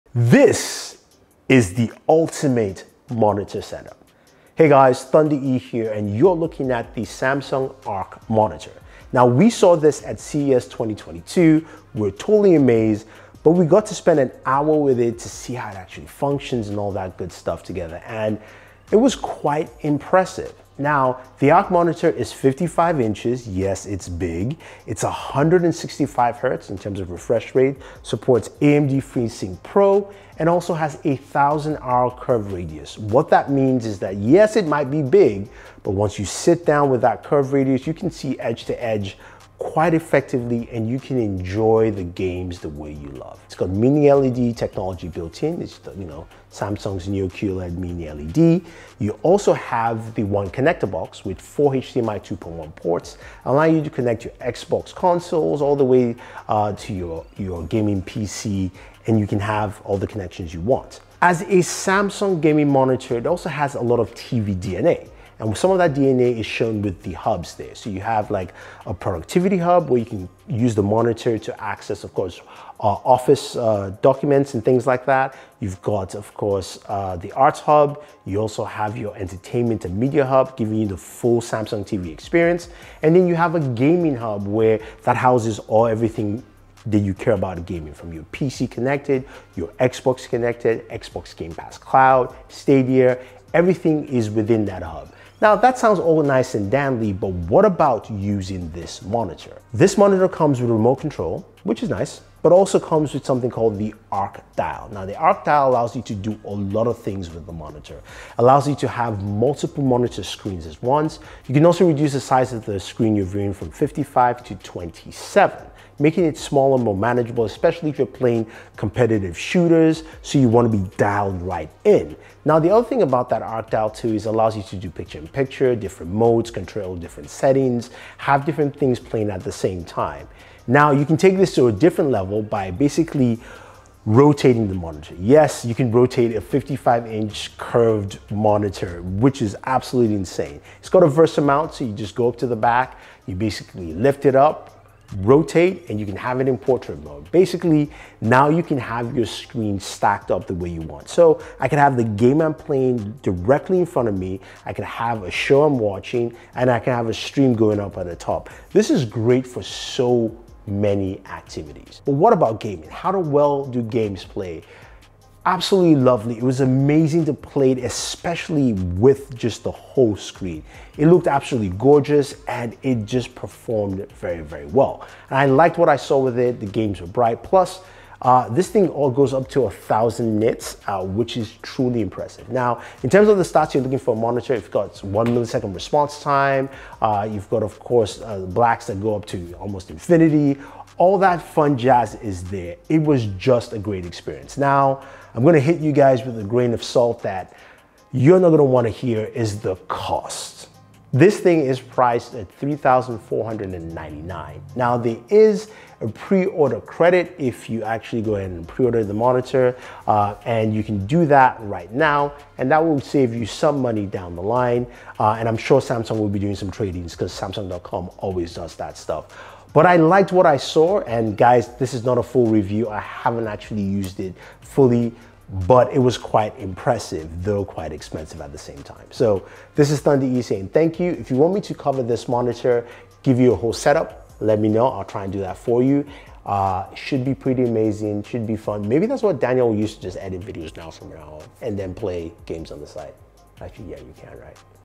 Samsung Odyssey ARK Gaming Monitor: 55-inches, 165Hz 4K MONSTER!!!!
The New Samsung Odyssey Ark Gaming Monitor is an insane 55" 4k Mini-LED 165hz Gaming Monitor that rotates. I mean Come on!